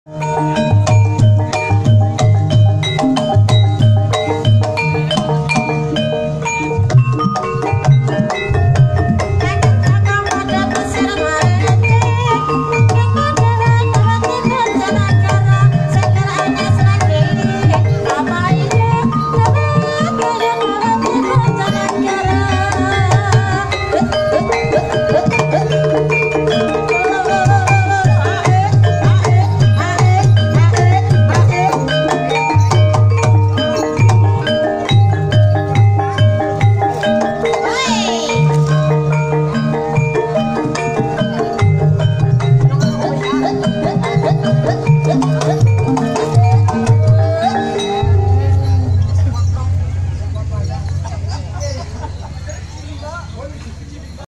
Ebeg KRIDA PERWIRA MUDA | bendrong kulon
Ebeg KRIDA PERWIRA MUDA
Festival Kesenian Tradisional Ebeg Banyumasan 2019
Karangpetir Kalimanah Purbalingga
10 Desember 2019